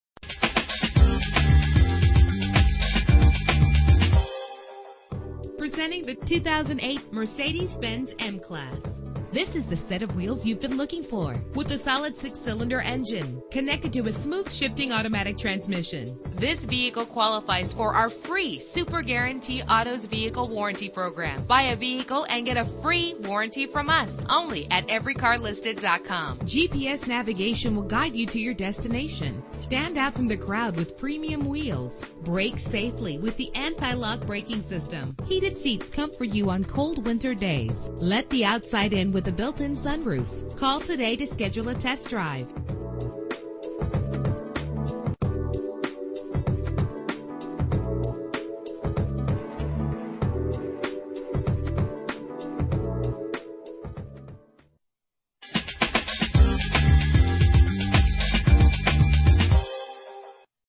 2008 Mercedes-Benz M-Class - Dublin OH
Year: 2008
Make: Mercedes-Benz
Model: M-Class
Trim: ML350 4MATIC
Engine: 3.5 liter 6 cylinder 24 valve
Transmission: 7-Speed Automatic
Color: Black
Mileage: 32243
Address:
6500 Perimeter Loop Rd